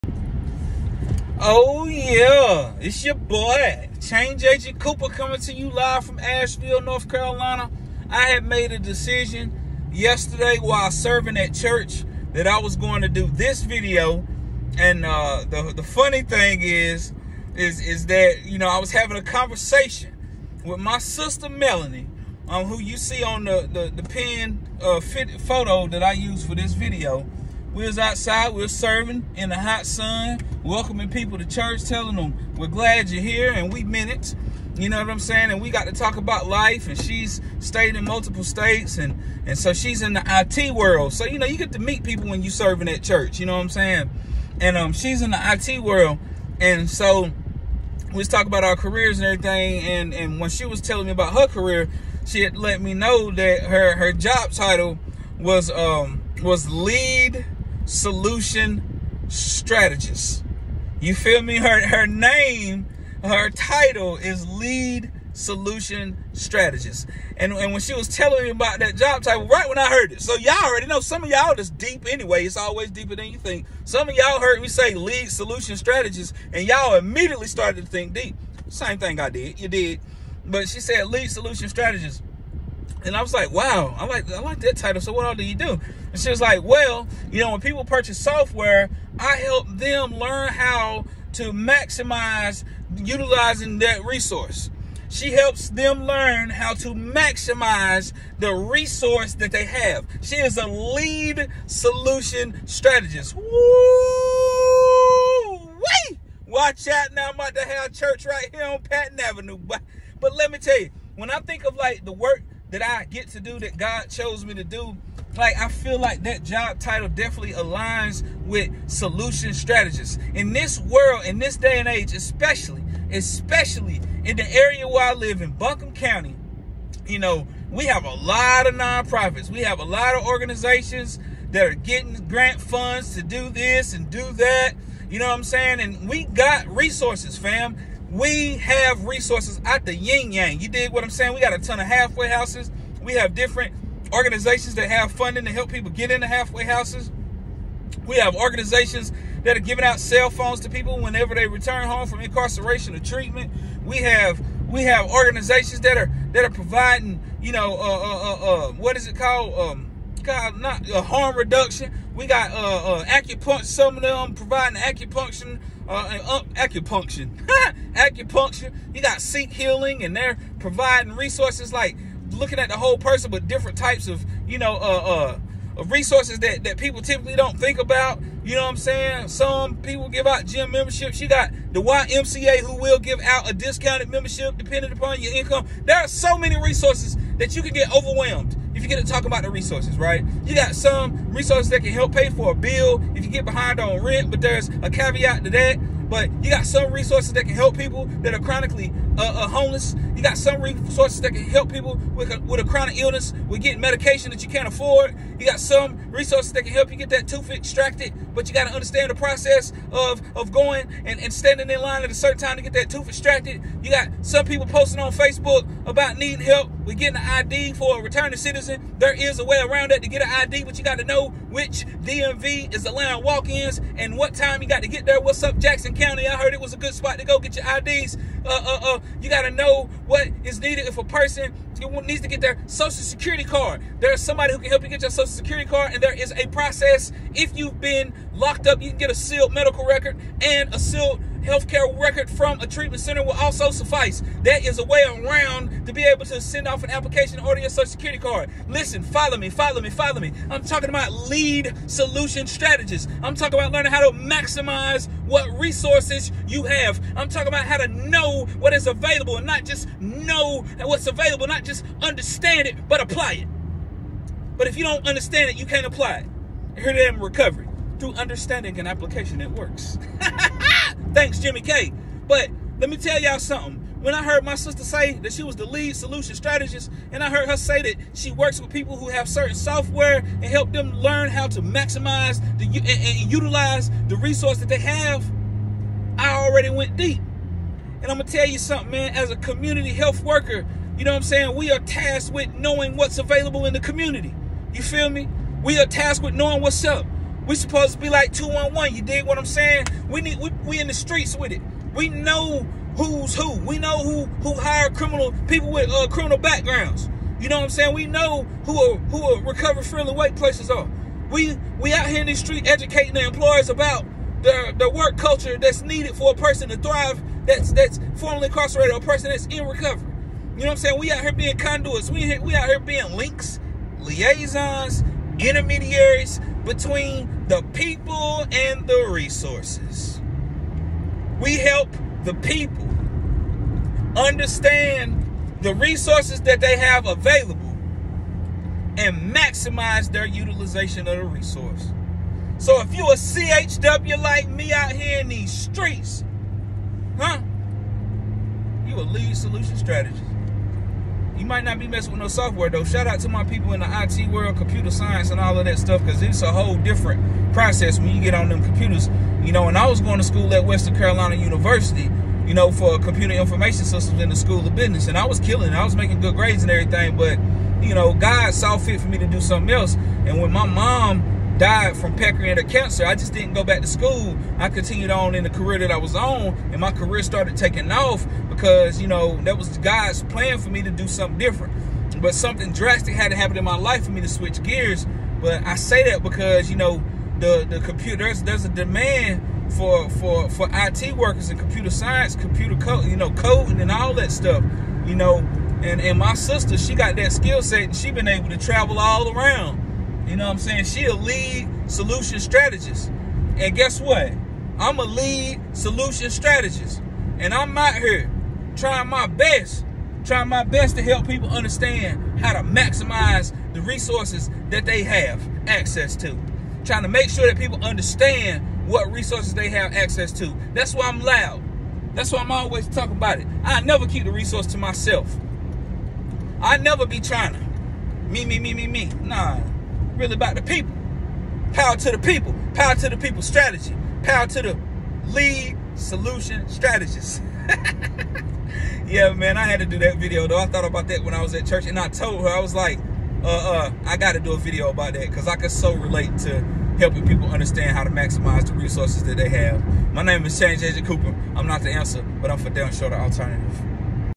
Lead Solution Strategist
My sister from church was telling me about her IT Career as a Lead Solution Strategist, which has provided opportunities for her to live in several different states. Pretty awesome, huh. But when she talked about her job, especially her job title, I couldn’t help but think about how there were correlations with the work that I do.

It’s all about solutions, baby!